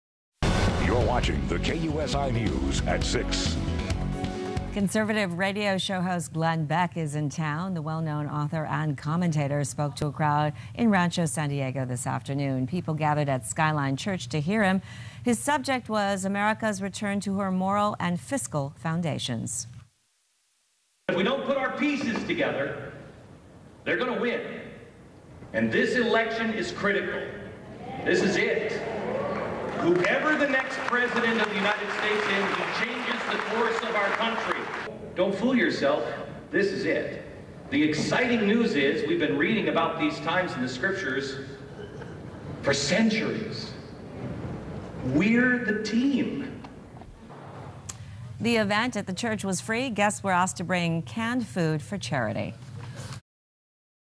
SKYLINE CHURCH WELCOMES GLENN BECK -- KUSI-TV 4-22-12 6 p.m.
On April 22,2012, Skyline Church held its first "Skyline Speaker Series" hosting Glenn Beck, along with co-authors of the New York Times Bestseller "INDIVISIBLE" by James Robison and Jay Richards. Over 3,500 guests filled the building throughout the day.